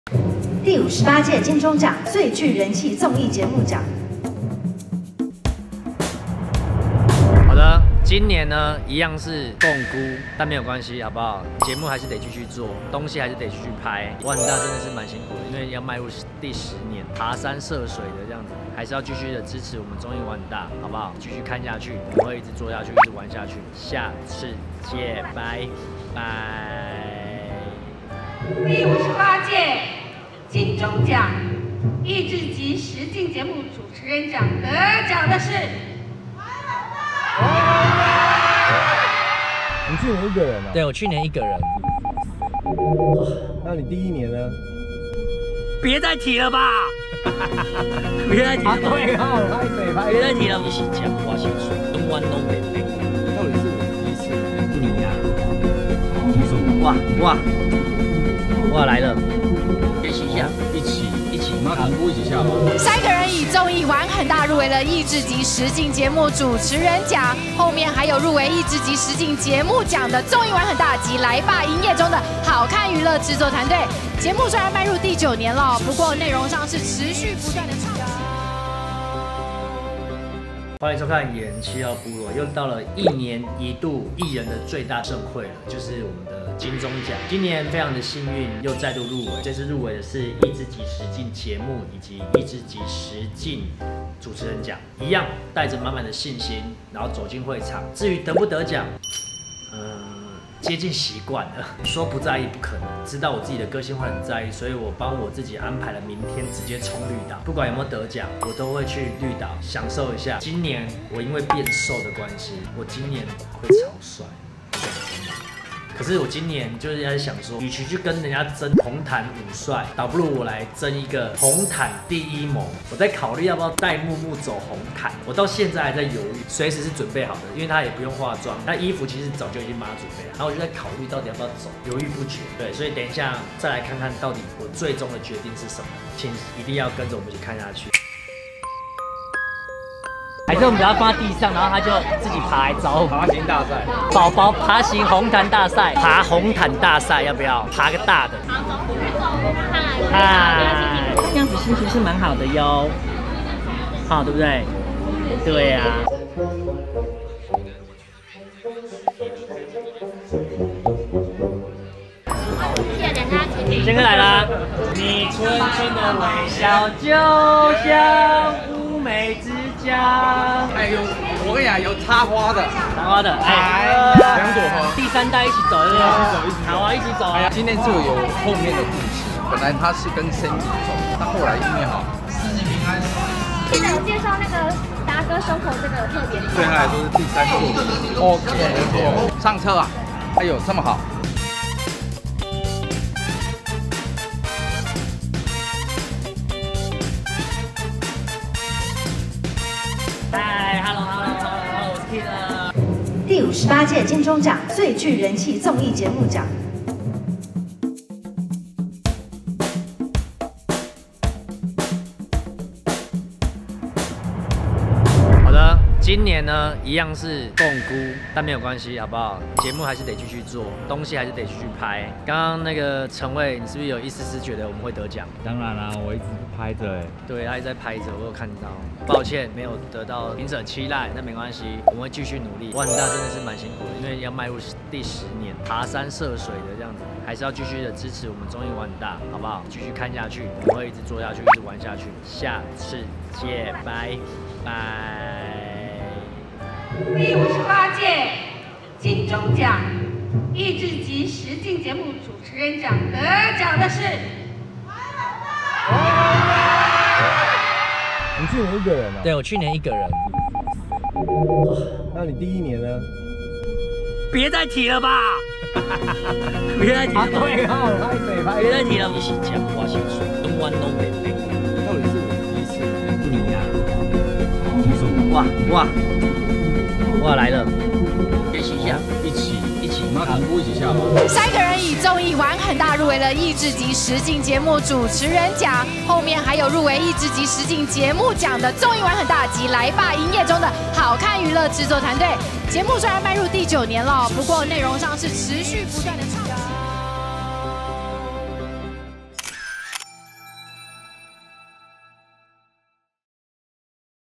KID沒有忘記小鬼 金鐘vlog藏洋蔥逼哭網｜三立娛樂星聞
野人七號部落 授權